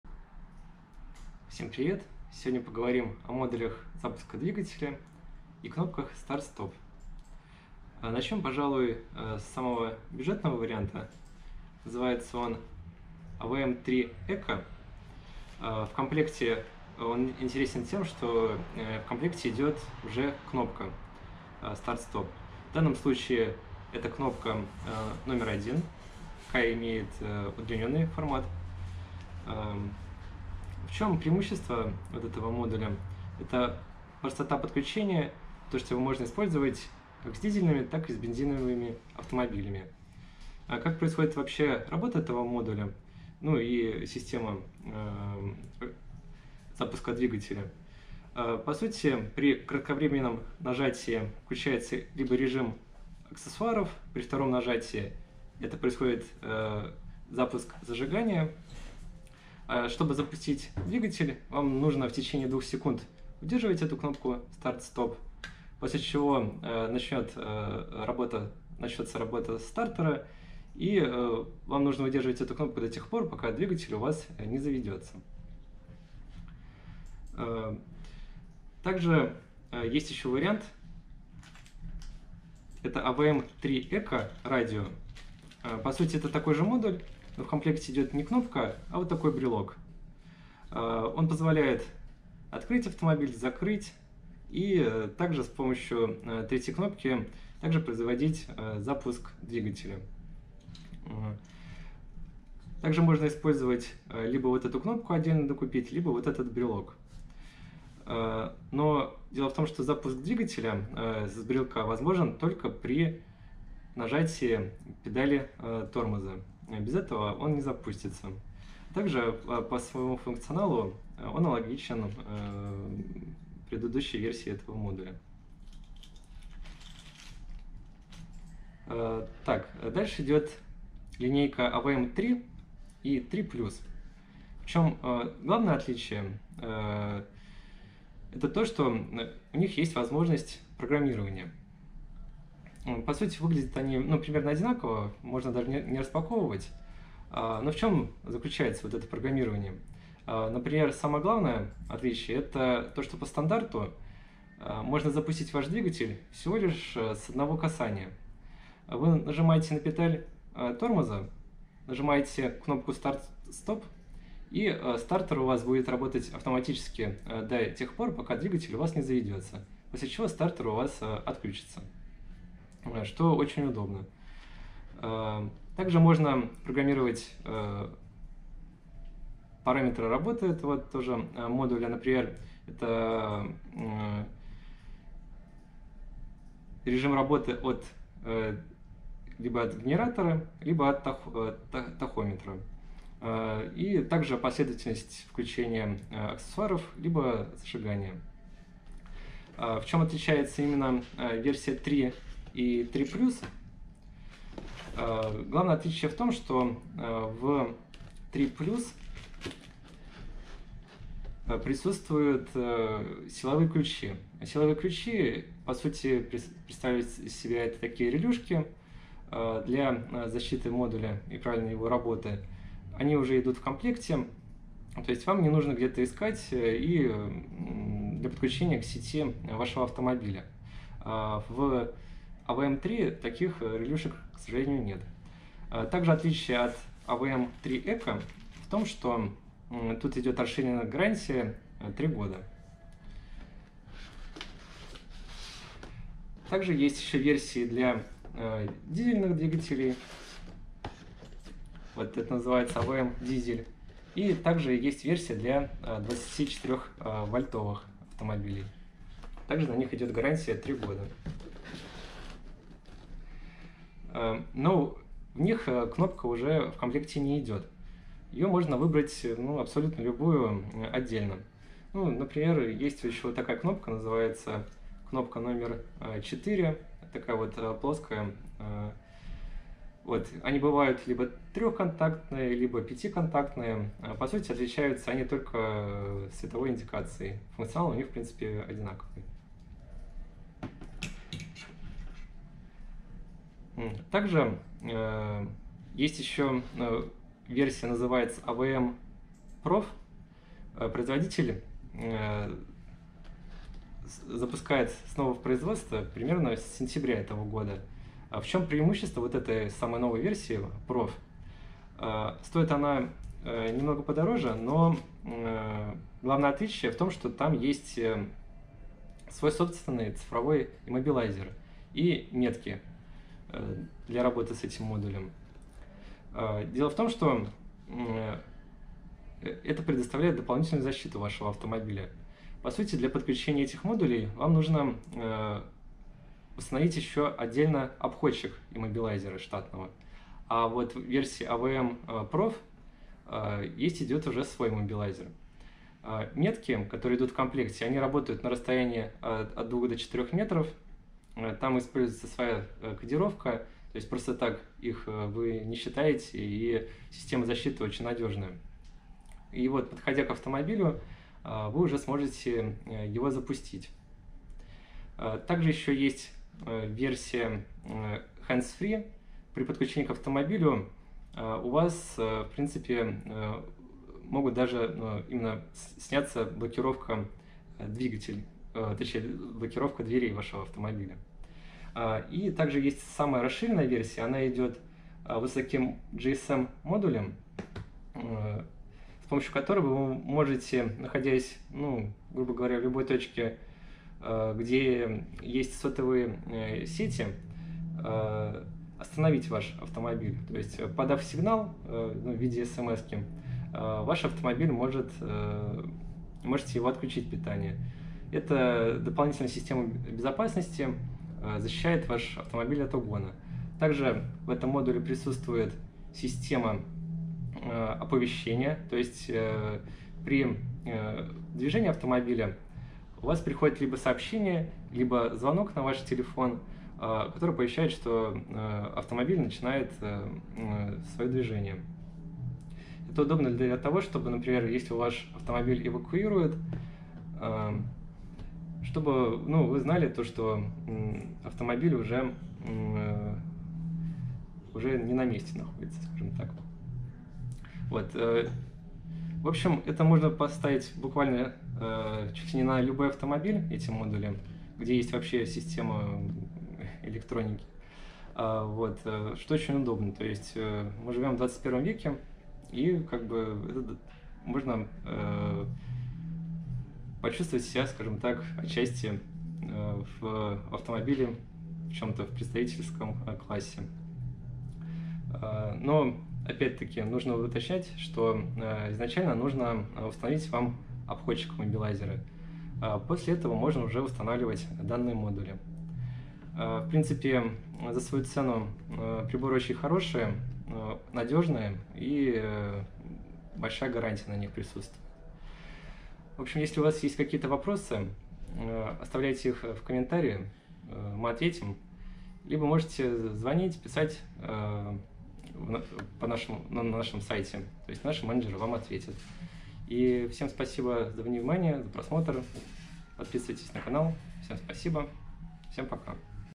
Модули запуска двигателя AVM
Сегодня рассмотрим модули запуска двигателя и кнопки Start-Stop

Кнопка "Engine Start Stop" v. №4